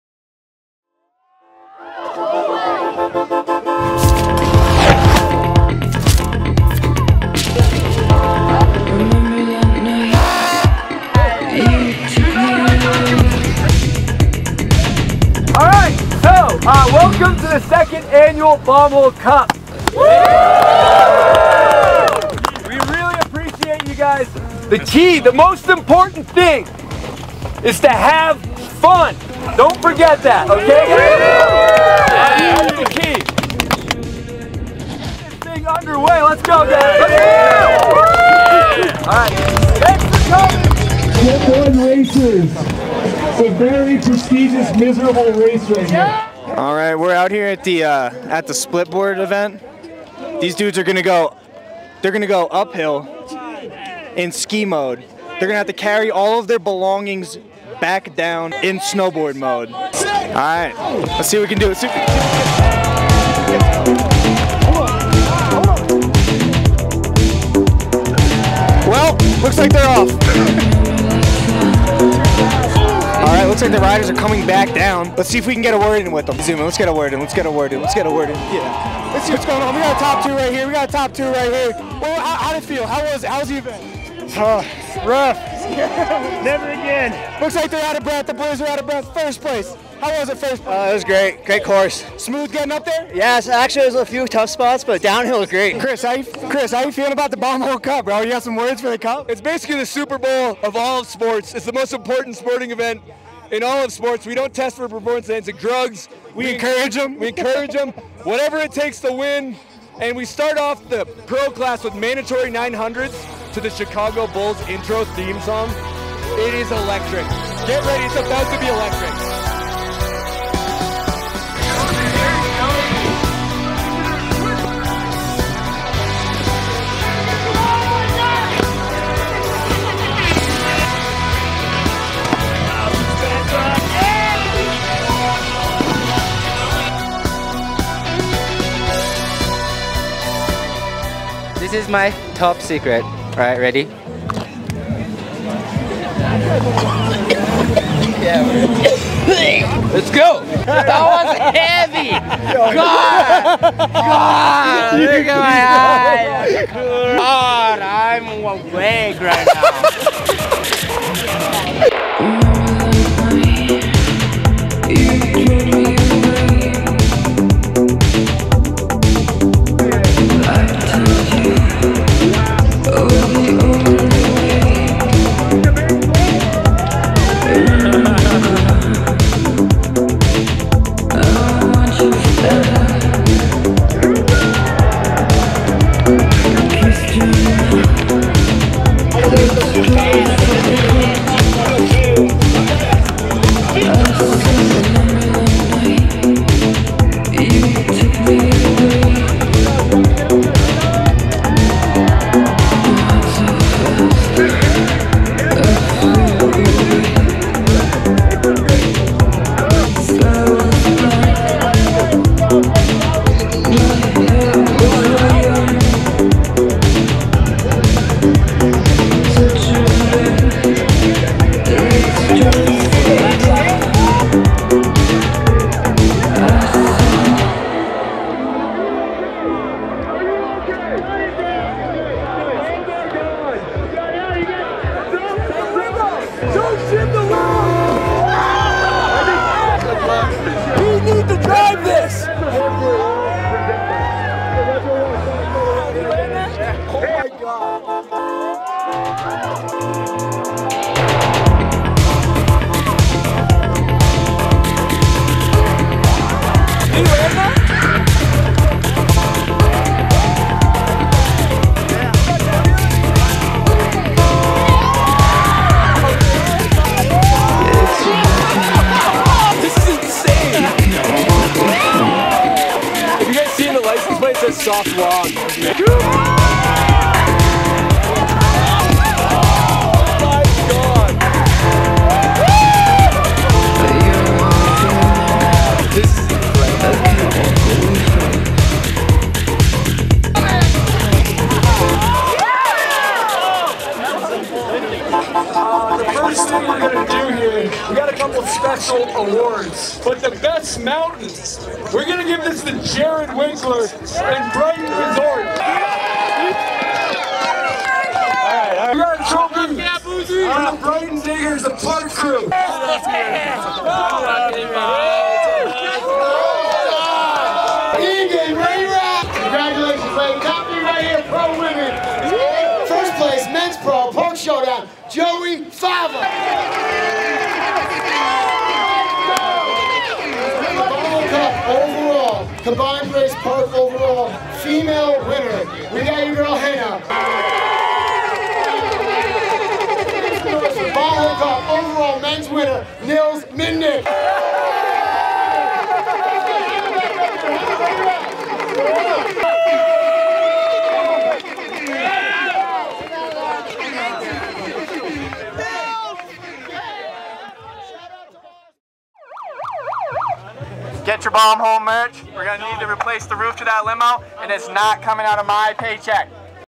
Bomb Hole Cup 2023 Recap
Recap of the 2023 Bomb Hole Cup at Brighton Resort. Thanks to our volunteers, participants, and sponsors Skullcandy, Sun Bum, Bubs Naturals, CBDayz, Wend Wax, Capita, Union, Forum, Red Bull and Save A Brain.

Filming and edit by Julian Matocq

Copyright Bomb Hole Media Inc. 2023